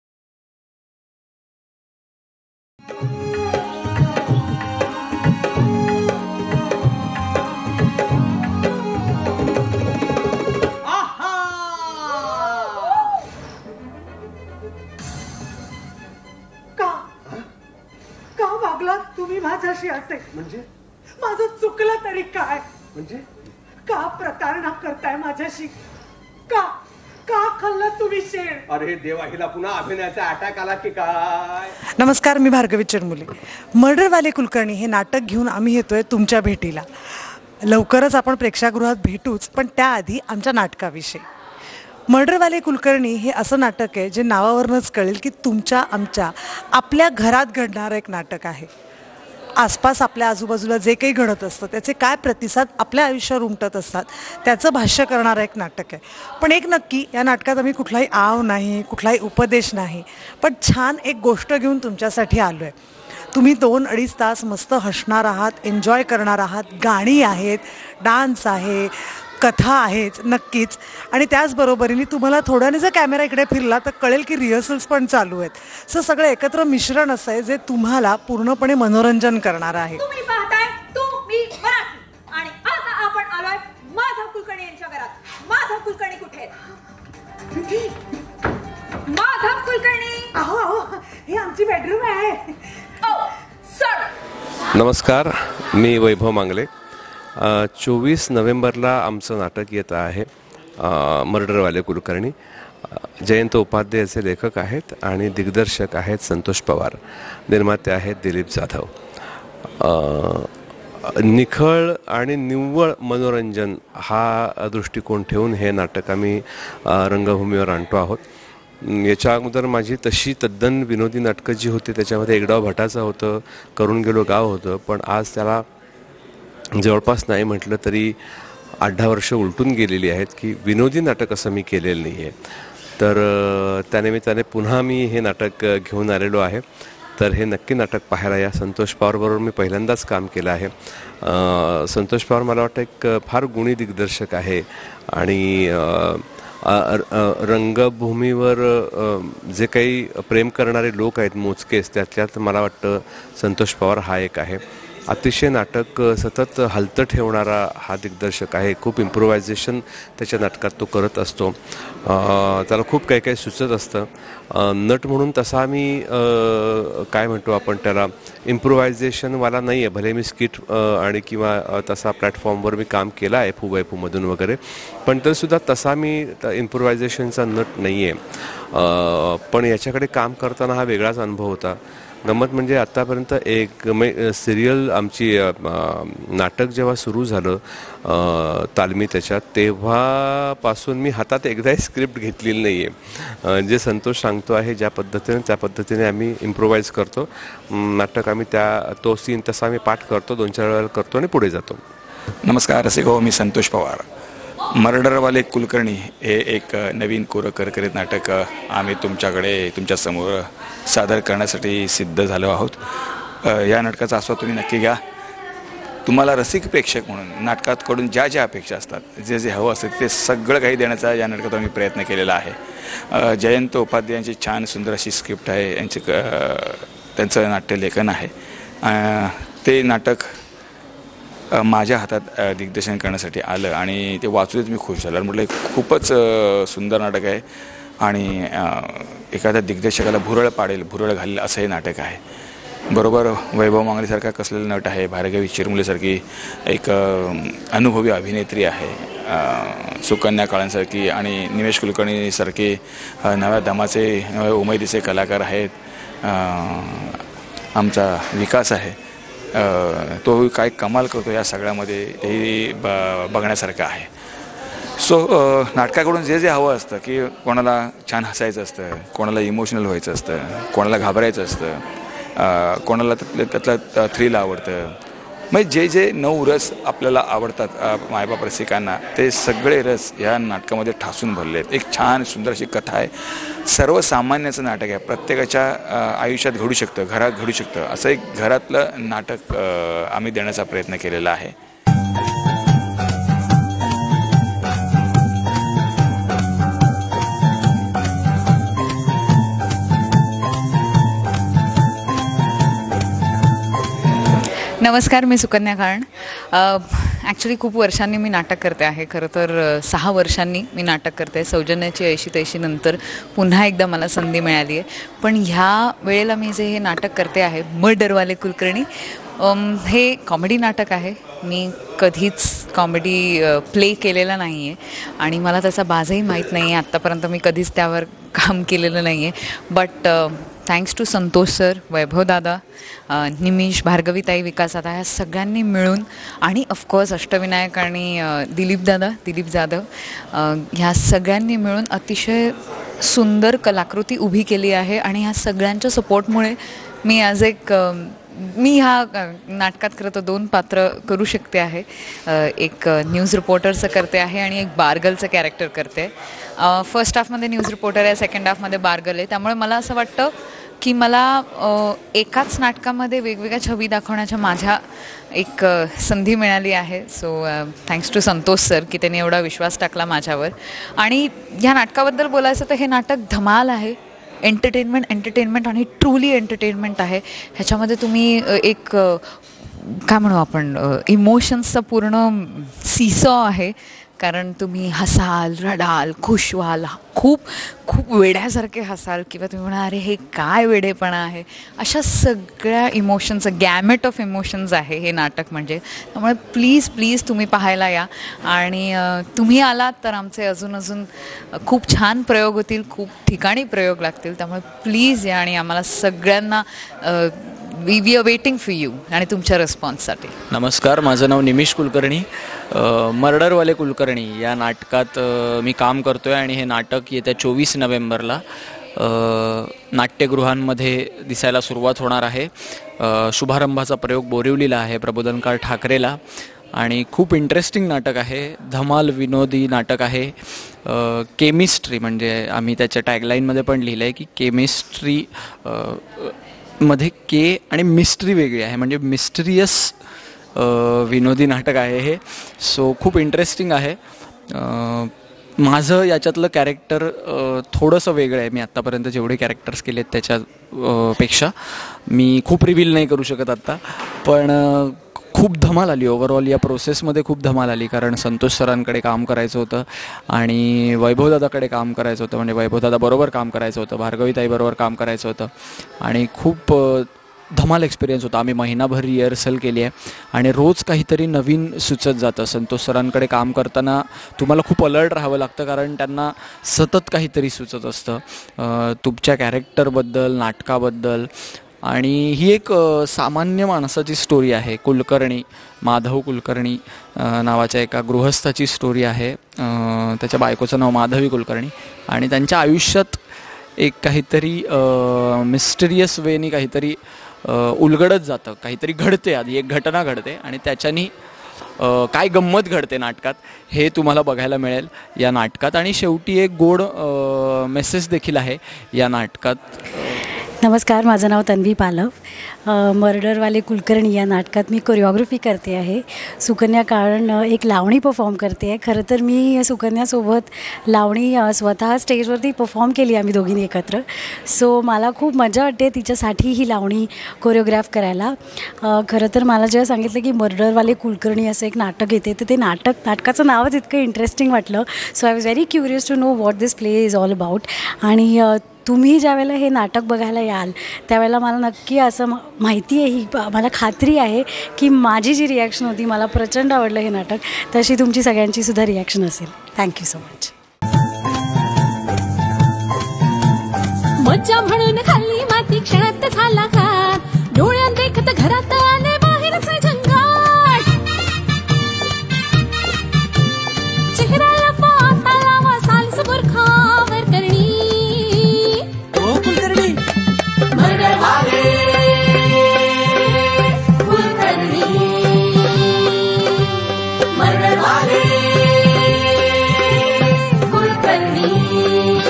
Murderwale Kulkarni Natak Video | Vaibhav Mangle | Bhargavi Chirmule | Santosh Pawar
Murderwale Kulkarni is a new full-on comedy marathi natak by santoshpawar with vaibhavmangle & bhargavichirmule. The Natak is a Murder mystery presented in the most hilarious and beautiful way.

00:00 Murderwale Kulkarni Natak Video
00:32 Bhargavi Chirmule Video Interview
01:20 Murderwale Kulkarni Natak Video
01:34 Vaibhav Mangle Video Interview
03:22 Santosh Pawar Video Interview
04:52 Murderwale Kulkarni Natak Video
05:05 Sukanya Kalan Video Interview
06:51 Nimish Kulkarni Video Interview
08:31 Tanvi Palav (Choreographer) Video Interview
09:11 Murderwale Kulkarni Natak Video